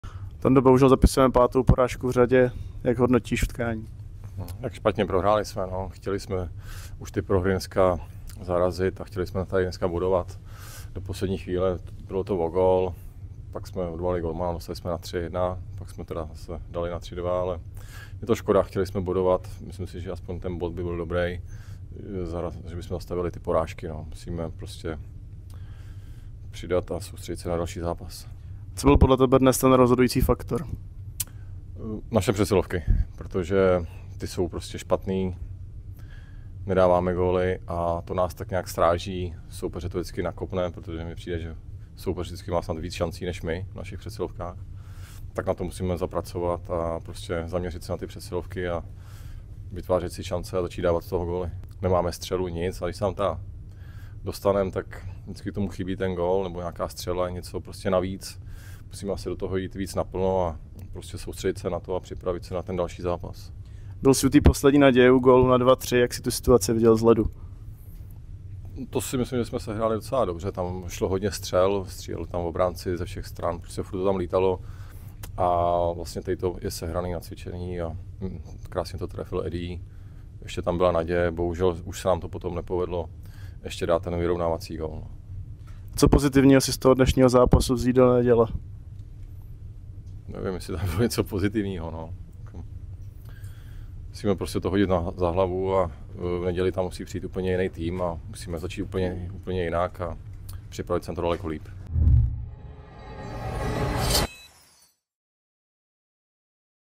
Antonín Melka: Je špatně když v přesilovce má soupeř více šancí, než my
První listopadový den zamířili Rytíři na půdu Komety Brno, odkud však odváží prohru 2:3. U posledního kladenského gólu byl na ledě i Antonín Melka: „Tam jsme to sehráli docela dobře, bylo tam hodně zakončení a Eddie to dobře trefil," popisuje situaci ze svého pohledu. Zamýšlí se také nad tím, že se opakovaně nedaří využívat přesilové hry: „Hrajeme je špatně a je to něco, co nás ráží, protože to ve finále vždy nakopne spíše soupeře."